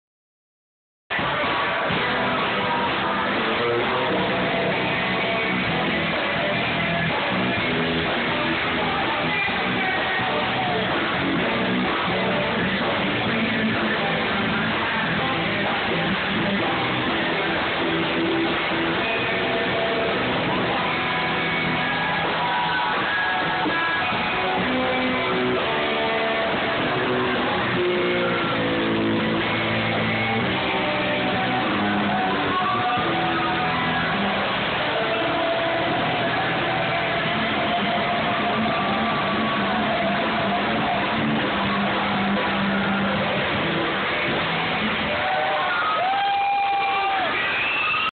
Banda KOSSA D3 en 12 ROUNDS Lounge & Sports Bar -2
Banda KOSSA D3 - Los veteranos del Rock en Español en el nuevo y más moderno lounge del área oeste de Puerto Rico: 12 ROUNDS Lounge & Sports Bar.

Localizado en el Combate Beach Resort, en la Carr. 3301, Km. 2.7 Playa El Combate, Cabo Rojo, Puerto Rico.